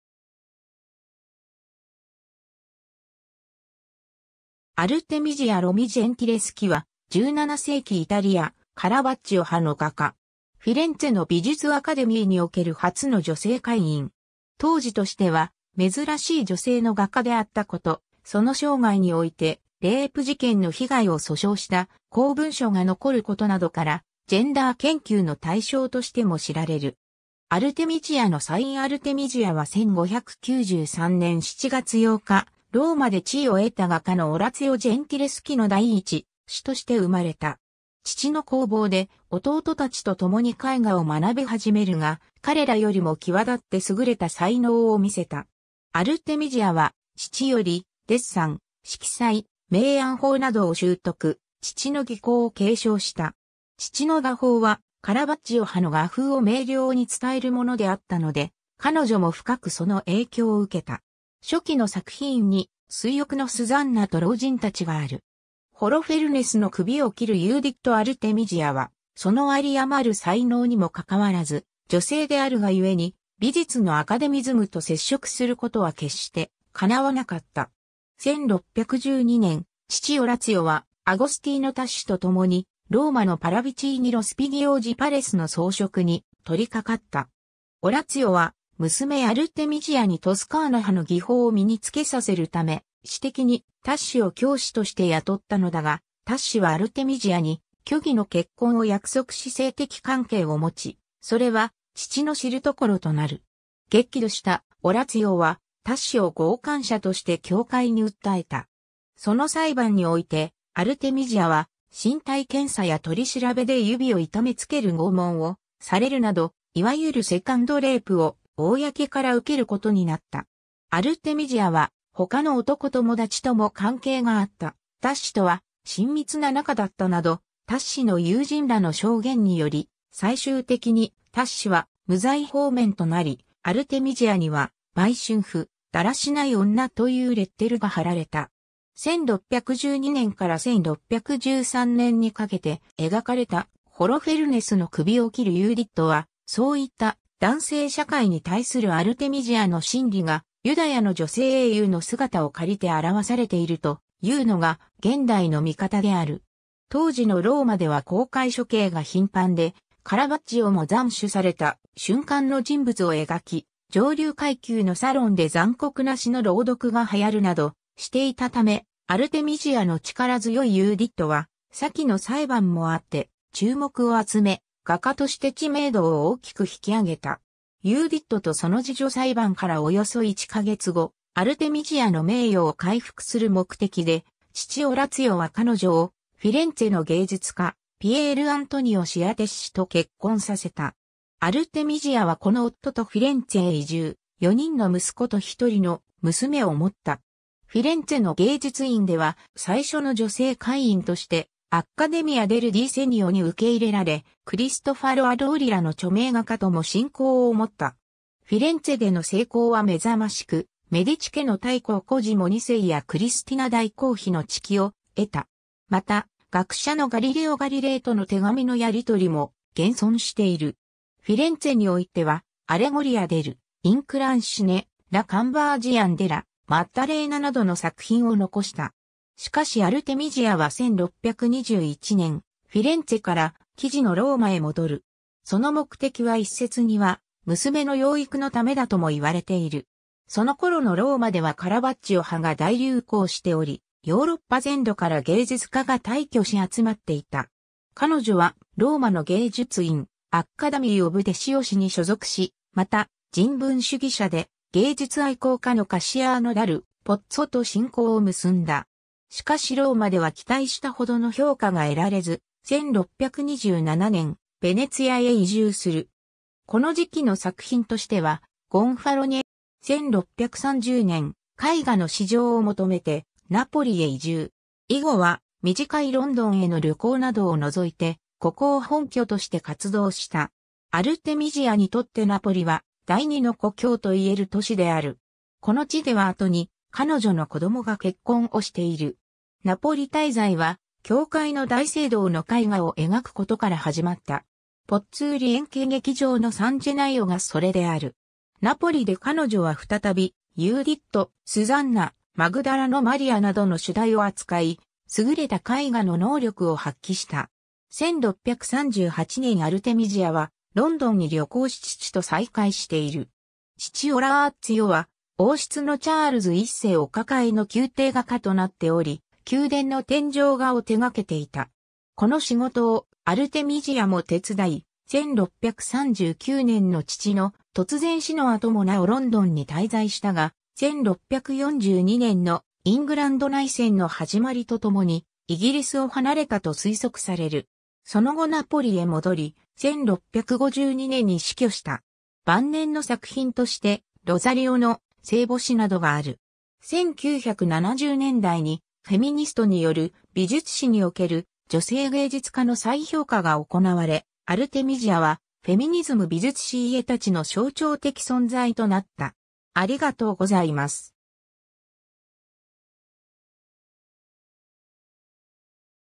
アルテミジア・ジェンティレスキ
アルテミジア・ロミ・ジェンティレスキ（Artemisia Lomi Gentileschi、1593年7月8日 - 1652年）は、17世紀イタリア、カラヴァッジオ派の画家。
フィレンツェの美術アカデミーにおける初の女性会員。
当時としては珍しい女性の画家であったこと、その生涯においてレイプ事件の被害を訴訟した公文書が残ることなどから、ジェンダー研究の対象としても知られる。
 アルテミジアのサイン アルテミジアは1593年7月8日、ローマで地位を得た画家のオラツィオ・ジェンティレスキ (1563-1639) の第一子として生まれた。
 父の工房で弟たちとともに絵画を学びはじめるが、彼らよりも際立って優れた才能をみせた。
アルテミジアは父よりデッサン、色彩、明暗法などを習得、父の技巧を継承した。
父の画法は、カラヴァッジオ派の画風を明瞭に伝えるものであったので、彼女も深くその影響をうけた。
初期の作品に『水浴のスザンナと老人たち』がある。
 『ホロフェルネスの首を斬るユーディット』 アルテミジアはそのあり余る才能にも拘らず、女性であるがゆえに美術のアカデミズムと接触することは決して叶わなかった。
1612年（一説には1611年）、父オラツィオはアゴスティーノ・タッシとともに、ローマのパラヴィチーニ・ロスピギオージ・パレスの装飾に取りかかった。
オラツィオは娘アルテミジアにトスカーナ派の技法を身につけさせるため、私的にタッシを教師として雇ったのだが、タッシはアルテミジアに虚偽の結婚を約束し性的関係をもち、それは父の知るところとなる。
激怒したオラツィオはタッシを強姦者として教会に訴えた。
その裁判において、アルテミジアは身体検査や取り調べで指をいためつける拷問をされるなど、いわゆるセカンド・レイプを公からうけることになった。
アルテミジアは他の男友達とも関係があった、タッシとは親密な仲だったなど、タッシの友人らの証言により、最終的にタッシは無罪放免となり、アルテミジアには「売春婦」、「だらしない女」というレッテルが貼られた。
1612年から1613年にかけて描かれた『ホロフェルネスの首を斬るユーディット』は、そういった男性社会に対するアルテミジアの心理が、ユダヤの女性英雄の姿を借りて表されているというのが現代の見方である。
当時のローマでは公開処刑が頻繁で、カラヴァッジオも斬首された瞬間の人物を描き、上流階級のサロンで残酷な詩の朗読が流行るなどしていたため、アルテミジアの力強いユーディットは先の裁判もあって注目を集め、画家として知名度を大きく引き上げた。
 ユーディットとその侍女 裁判からおよそ1か月後、アルテミジアの名誉を回復する目的で、父オラツィオは彼女をフィレンツェの芸術家、ピエール・アントニオ・シアテッシと結婚させた。
アルテミジアはこの夫とフィレンツェへ移住、4人の息子と1人の娘（こどもの人数については諸説あり）を持った。
フィレンツェの芸術院では最初の女性会員としてAccademia del Disegno （絵画アカデミー）に受け入れられ、クリストファロ・アッローリらの著名画家とも親交をもった。
フィレンツェでの成功はめざましく、メディチ家の大公コジモ2世やクリスティナ大公妃の知己を得た。
また、学者のガリレオ・ガリレイとの手紙のやりとりも現存している。
フィレンツェにおいては Allegoria dell'Inclinazione、La conversione della Maddalena（ピッティ宮所蔵）などの作品を残した。
 しかしアルテミジアは1621年、フィレンツェから生地のローマへ戻る。
その目的は一説には娘の養育のためだともいわれている。
その頃のローマではカラヴァッジオ派（カラバジェスキ）が大流行しており、ヨーロッパ全土から芸術家が大挙し集まっていた。
 彼女はローマの芸術院Accademy of Desiosiに所属し、また人文主義者で芸術愛好家のカッシアーノ・ダル・ポッツォと親交を結んだ。
しかしローマでは期待したほどの評価が得られず、1627年ヴェネツィアへ移住する。
この時期の作品としては『ゴンファロニエーレの肖像』や『ユーディットと侍女』、『エステルとアハシュエロス』、『眠れるヴィーナス（ヴィーナスとキューピッド）』...